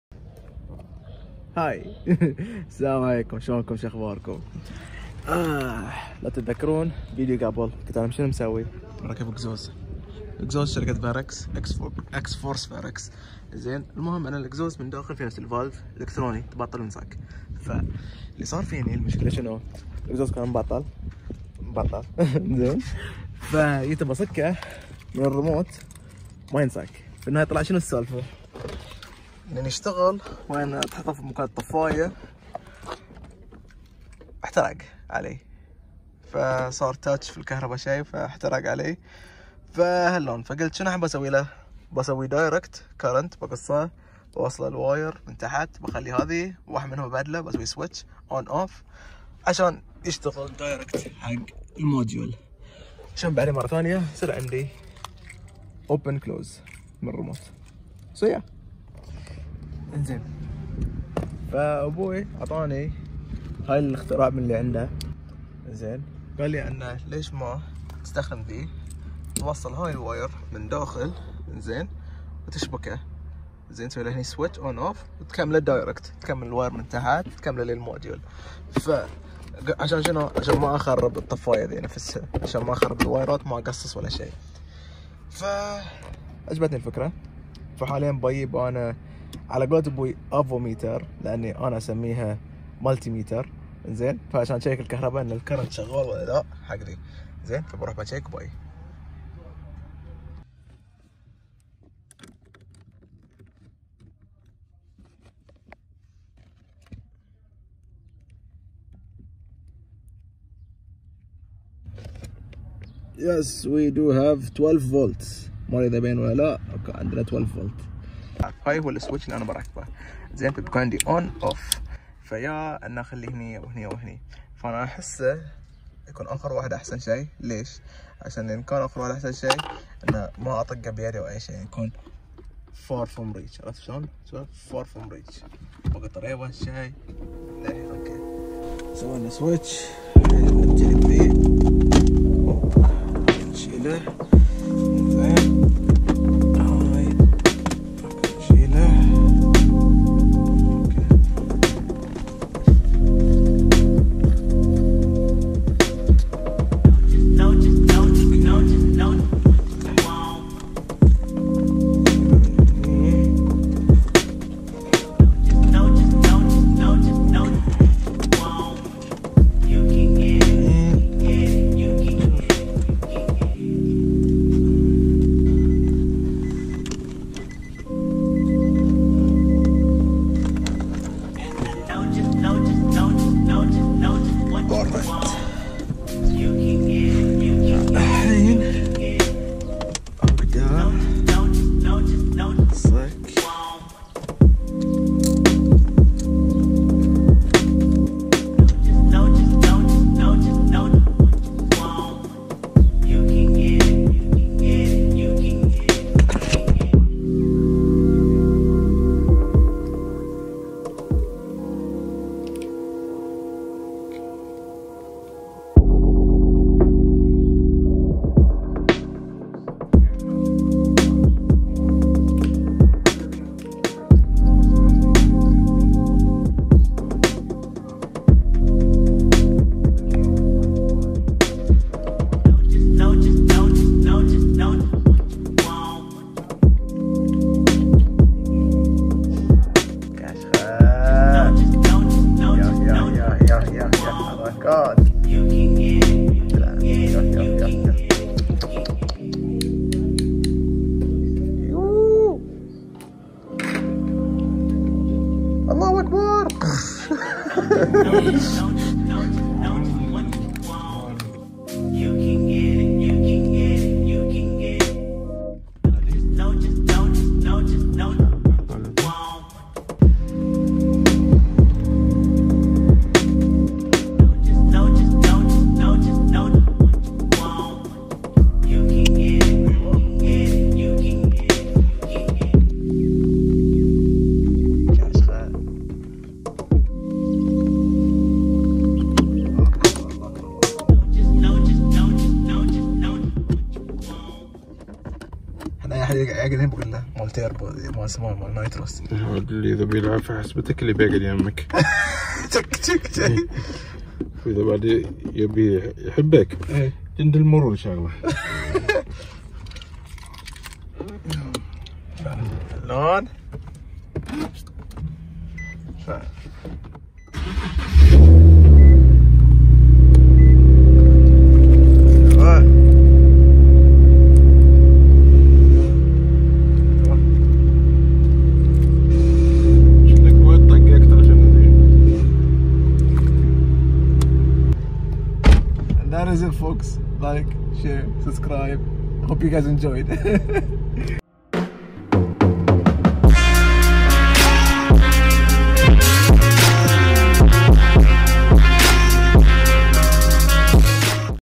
varex xforce exhust swtich 370z
for any questions feel free to dm me or post it in the comments below

items used
12v switch
soldiering iron
avometer or multimeter